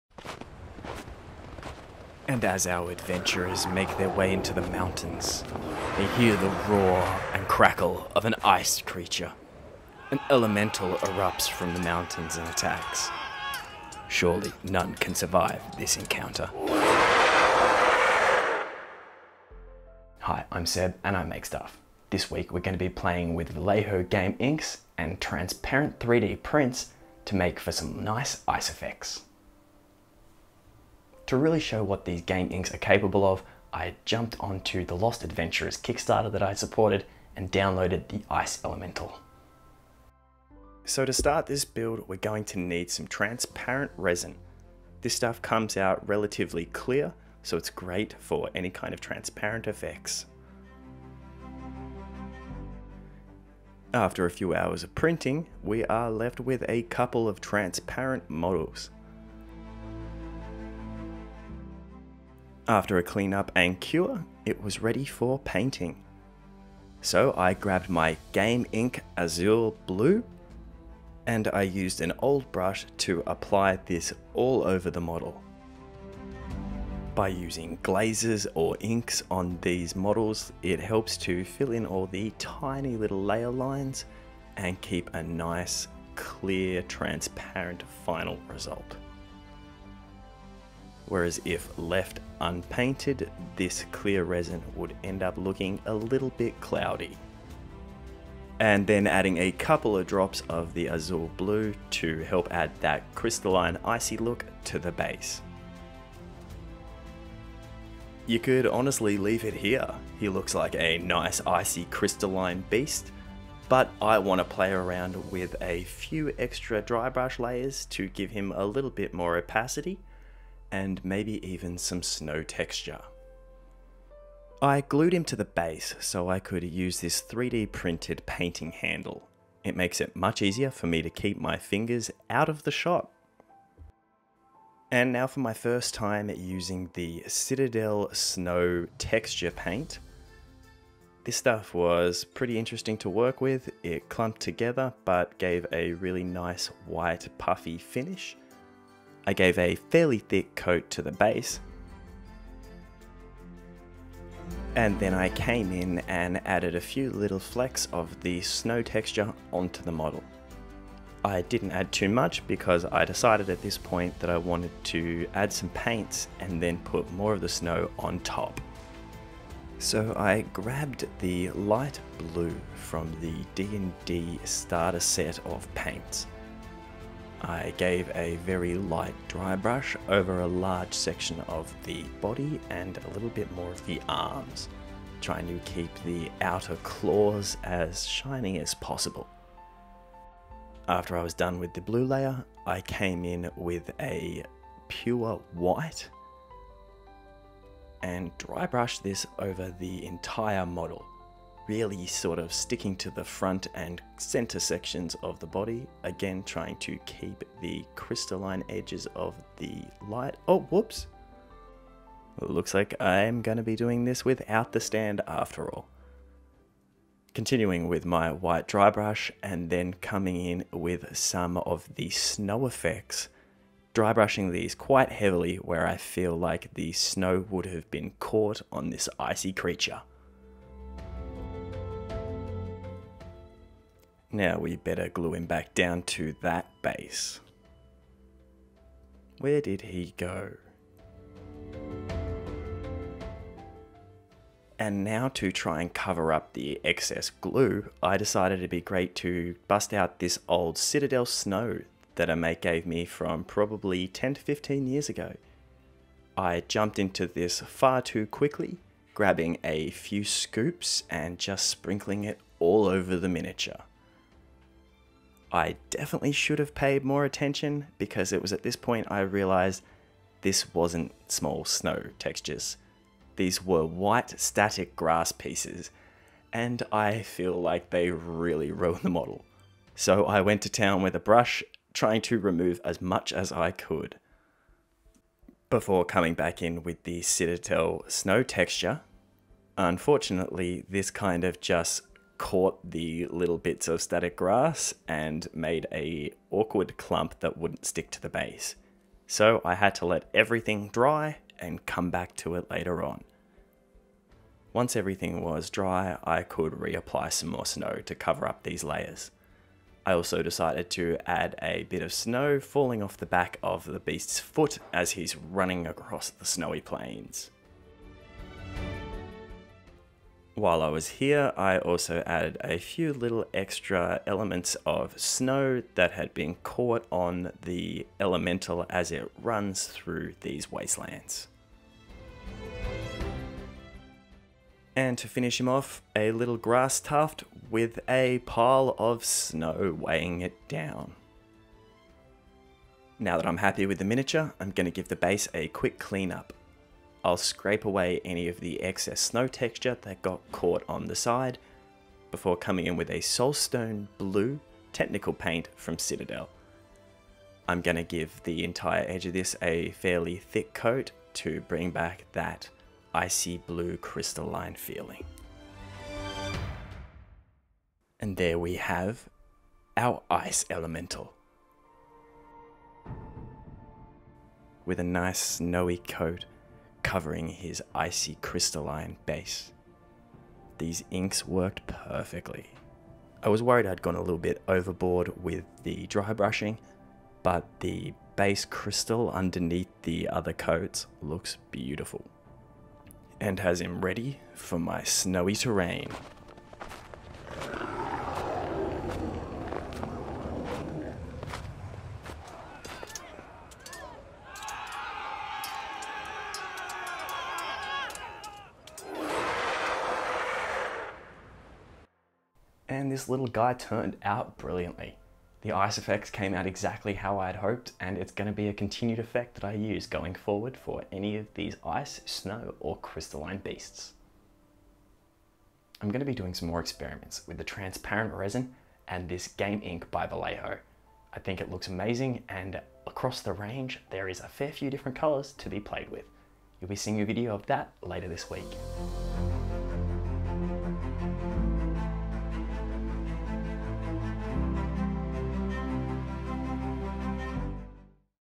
The ICE ELEMENTAL | Painting a Frosty Ice Effect
How to Paint An Ice Effect on Clear Resin

Using Anycubic Clear Resin to print an Ice Elemental from the Lost Adventurers Kickstarter. Then Combining classic paints with a base coat of the Vallejo Game Ink Blue.

Seb Makes Stuff Socials - I would love to see your builds!

chapters
00:00 intro
00:32 Getting my Transparent Models
01:06 Using the Game Inks
02:15 Citadel Snow texture base
02:35 Painting the Ice Effects
04:05 DON’T USE THIS PRODUCT
04:40 Fixing my Mistake
05:20 Finishing Touches
06:07 The Ice Elemental - Hero Shots